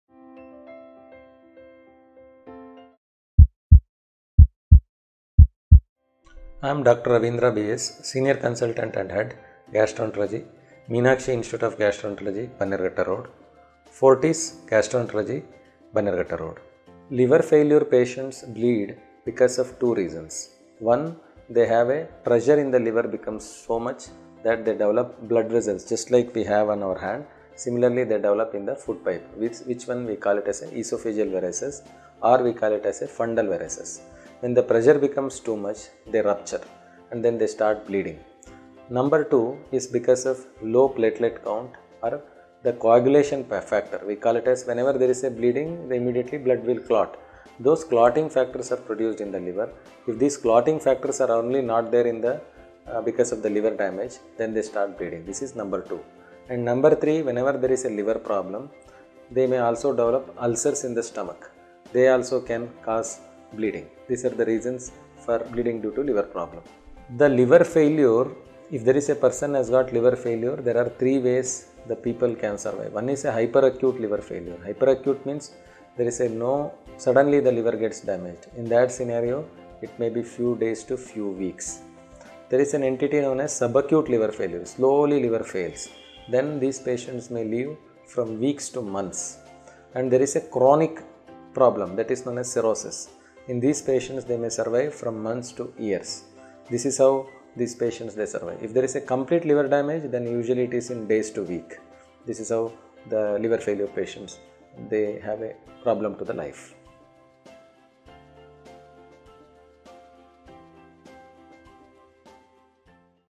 Why do liver failure patients bleed?Can You Live With Liver Failure?-Dr. Ravindra BS|Doctors' Circle
Dr Ravindra BS | Appointment Booking No. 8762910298
Consultant Gastroenterologist & Hepatologist | Meenakshi Gastro Liver Care
Liver failure patients bleed  because  of  two  reason one is  because of   pressure in the liver becomes so much that  they develop  so much  blood  vessels  like the ones we have  on the hand,  similarly they develop in the food pipe, which  we  call as  esophageal varices or fundal  varices. When the pressure becomes too much, they rupture and start  bleeding.  Number two  is because of  low platelet  count and because of coagulation factor. Whenever there is a bleeding,